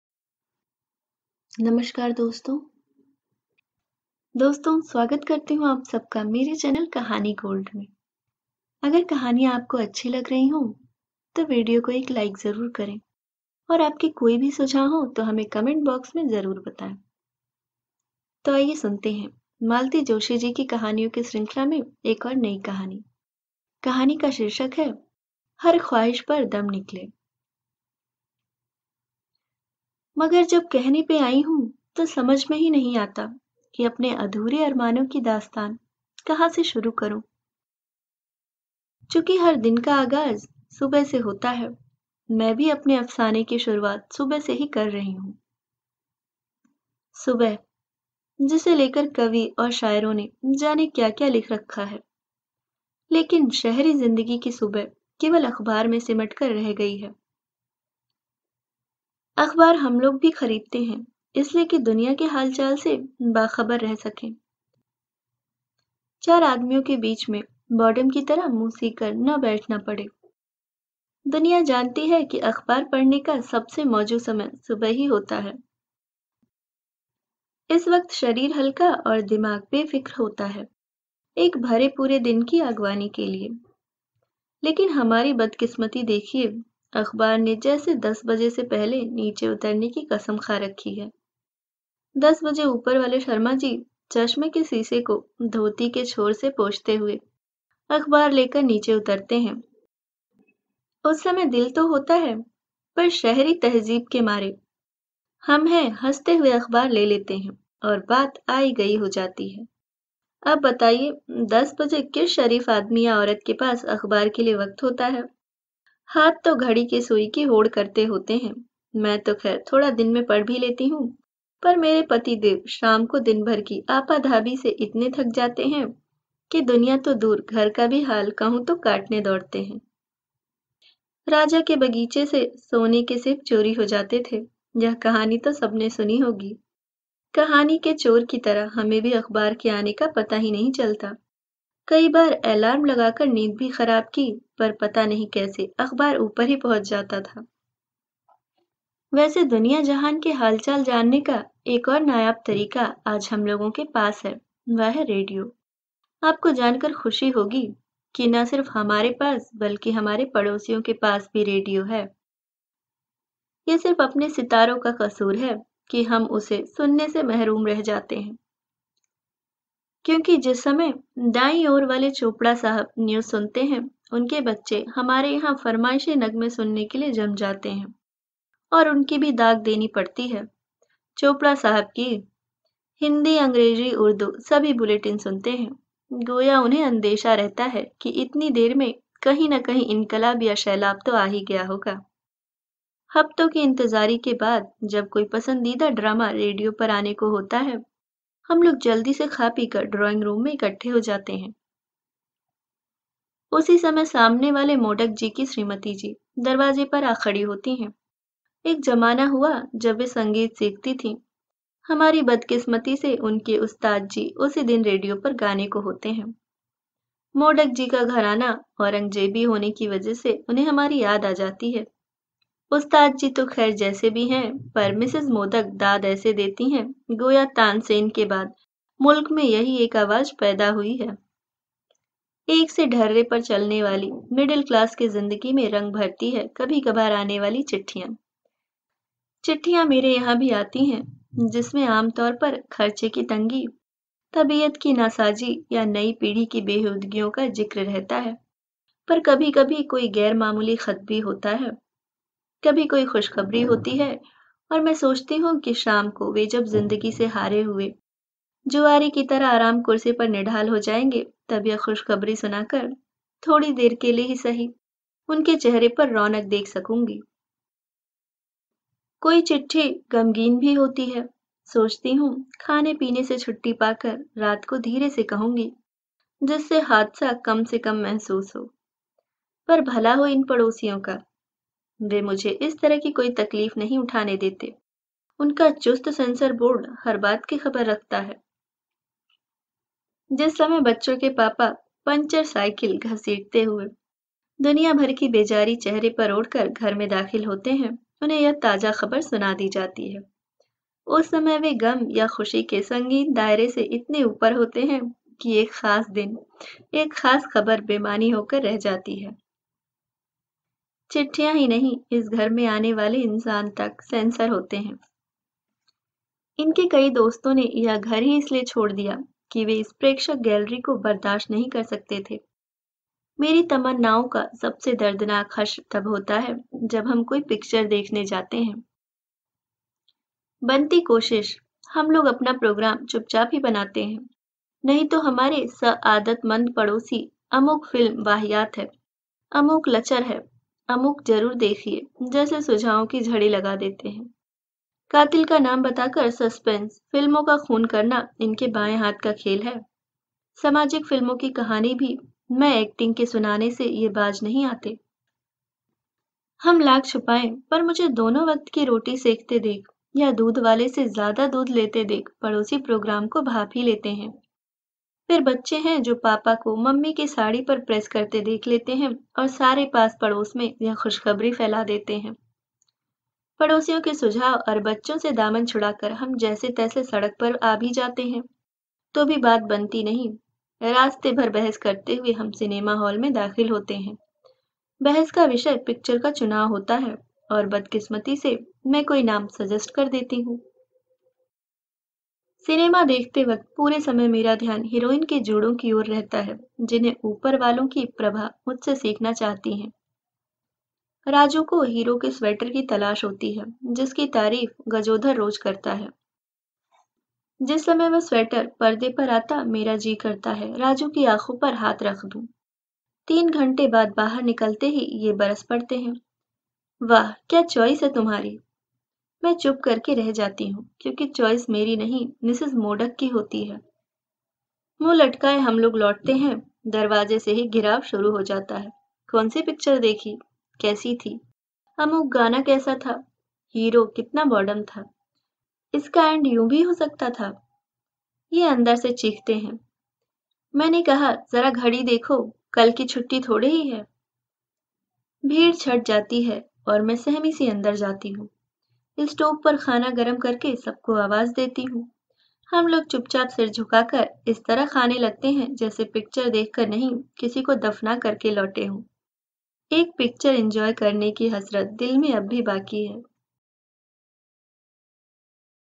Malti Joshi Story Har Khwahish Par Dam Nikale | हर ख्वाहिश पर दम निकले | मालती जोशी
Malti Joshi Story -हर ख्वाहिश पर दम निकले |

Narrator - Ruchika || कथा-वाचक - रुचिका

मालती जोशी एक हिन्दी लेखिका हैं, जिन्हें 2017 में साहित्य तथा शिक्षा के क्षेत्र में उनके योगदानों के लिए पद्म श्री से सम्मानित किया गया।

मालती जोशी का जन्म 4 जून 1934 को औरंगाबाद में हुआ था। आगरा विश्वविद्यालय से  हिन्दी विषय से एम.ए. की शिक्षा ग्रहण की। अब तक अनगिनत कहानियां, बाल कथायें व उपन्यास प्रकाशित कर चुकी हैं। इनमें से अनेक रचनाओं का विभिन्न भारतीय व विदेशी भाषाओं में अनुवाद भी किया जा चुका है। कई कहानियों का रंगमंचन रेडियो व दूर दर्शन पर नाट्य रूपान्तर भी प्रस्तुत किया जा चुका है। कुछ पर जया बच्चन द्वारा दूरदर्शन धारावाहिक सात फेरे का निर्माण किया गया है तथा कुछ कहानियां गुलज़ार के दूरदर्शन धारावाहिक किरदार में तथा भावना धारावाहिक में शामिल की जा चुकी हैं। इन्हें हिन्दी व मराठी की विभिन्न व साहित्यिक संस्थाओं द्वारा सम्मानित व पुरस्कृत किया जा चुका है। मध्य प्रदेश हिन्दी साहित्य सम्मेलन द्वारा वर्ष 1997 के भवभूति अलंकरण सम्मान से विभूषित किया जा चुका है। मालती जोशी जी कि कहानियां मन को छूने वाली होती हैं।